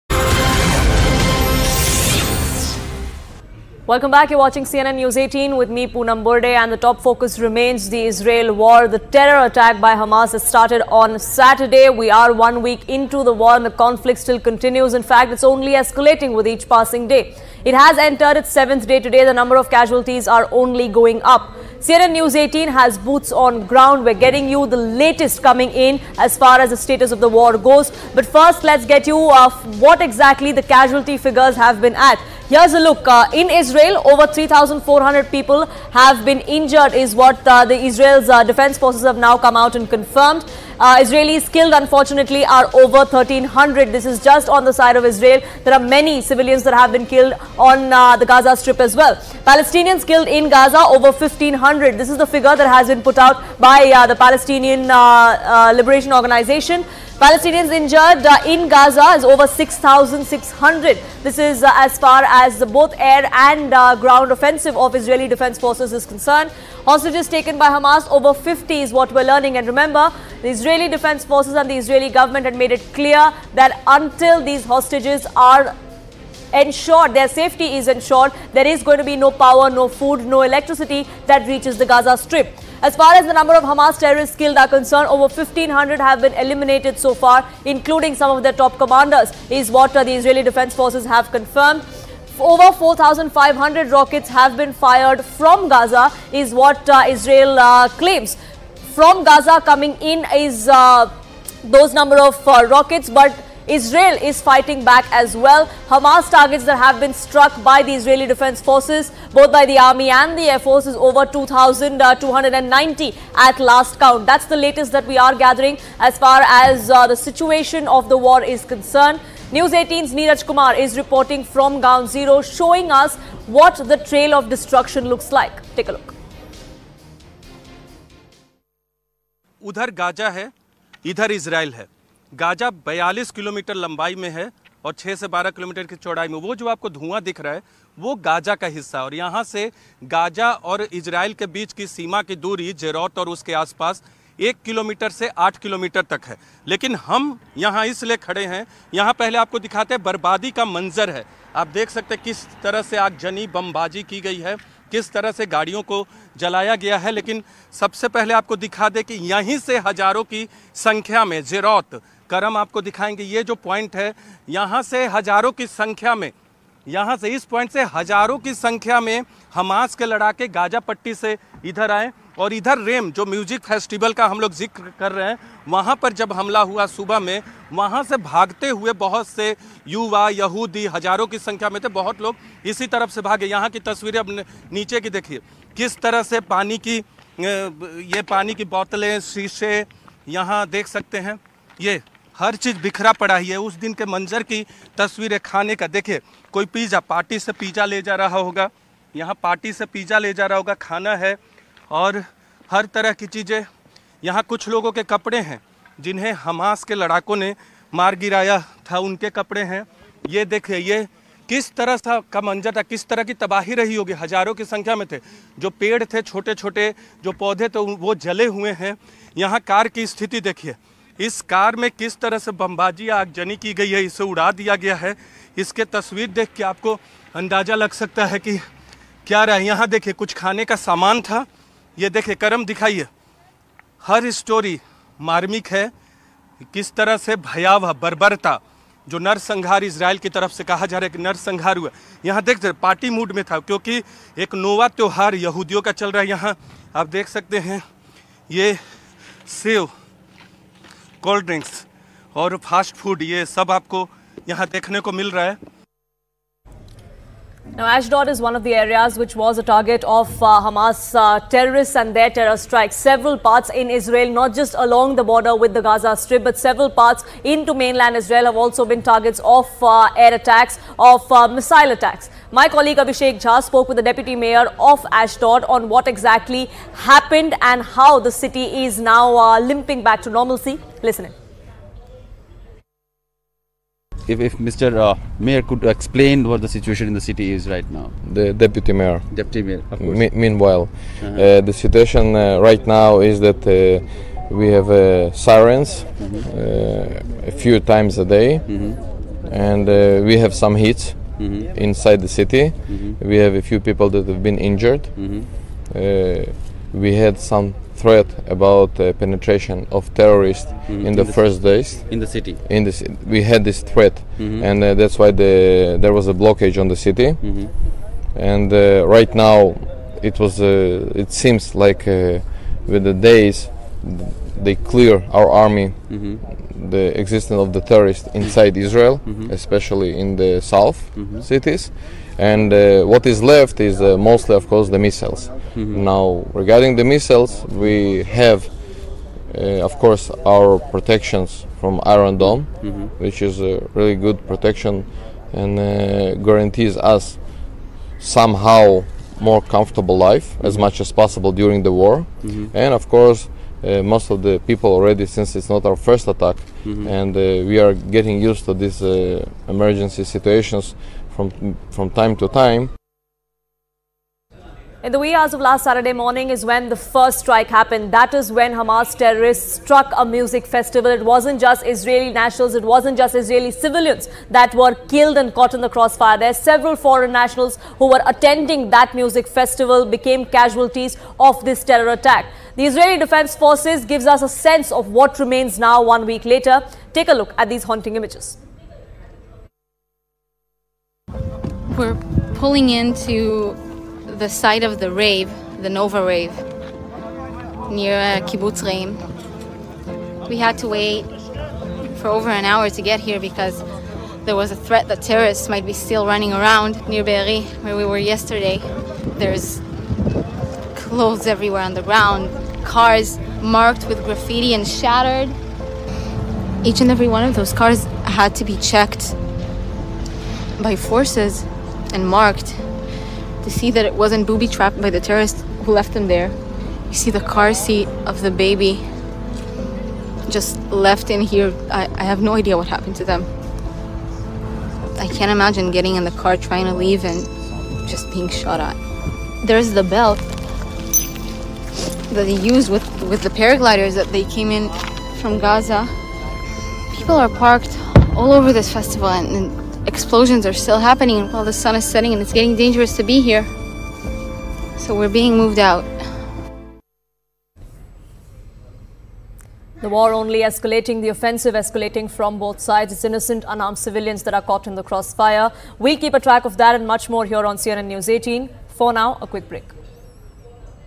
Israel-Hamas War: Over 1300 Killed, More Than 3400 Injured In Israel | Palestine-Israel News
More than 1,300 people have been killed in Israel since Hamas militants unleashed a massive attack from the blockaded Gaza Strip, the army said Friday, raising an earlier toll by 100. Over 3,200 other people had been wounded, the Israeli army said on X, formerly Twitter, while families of 120 hostages have been contacted since attack at dawn on Saturday that spiralled into all-out war.

n18oc_business